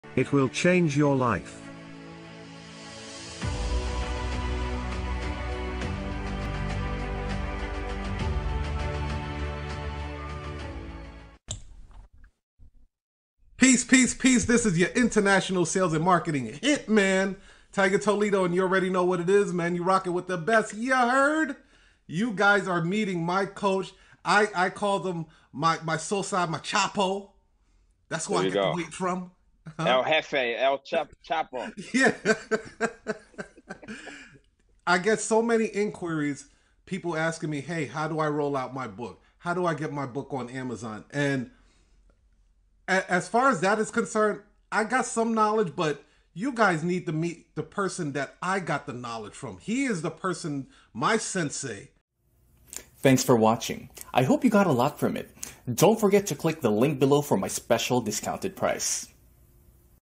Episode 18: The Notary War Room Live (Guest: Ty Cohen of Kindle CashFlow)
Featured in Black Enterprise, NBC, FOX, CBS, and FORBES
This is a Big Deal so continue reading.

I'm often asked by different notaries and entrepreneurs how do I publish my books?

How do I get my book out to the marketplace?

I try to give them the best advice I can but in this *New Notary War Room episode I will be introducing my coach.

The person that actually got me into the book space to be a 3X Best-Selling author with over 28 books in my armory, Mr. Ty Cohen.

You don't want to miss this episode of The Notary War Room Live

Special Invited Guest: Ty Cohen

Follow Ty Cohen at:
KindleCashFlow.com
KCFLive.com